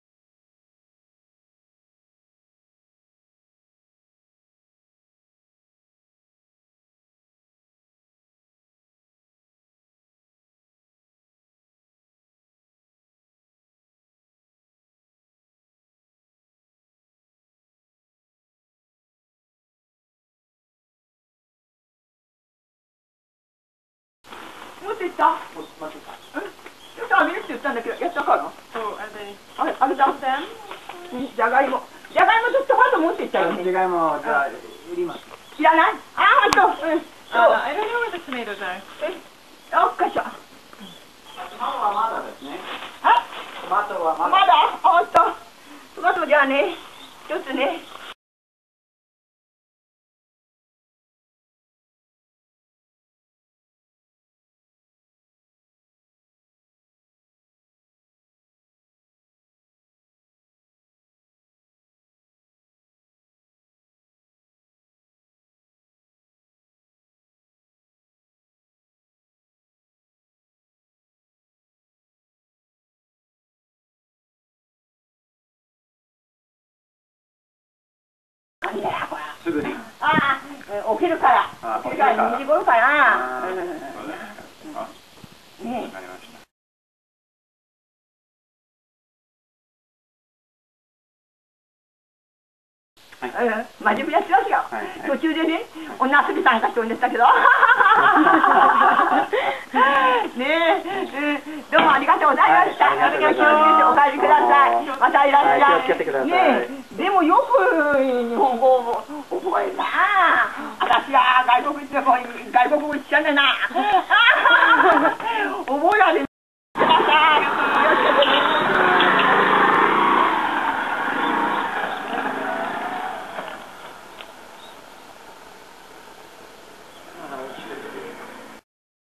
Motor scooter veggie mama!
A short video of a scooter-riding farmer out selling produce